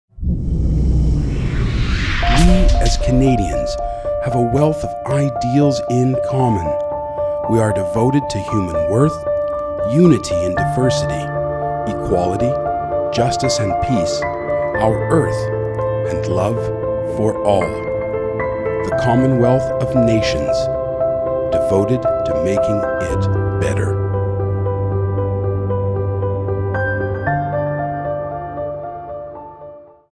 Devoted to Making it Better | The Commonwealth of Nations | English Version
Educational public service announcement about the Commonwealth of Nations.

Produced by Absolut Media.
Commissioned by the Edmonton Branch of the Royal Commonwealth Society.
htttp://rcs.ca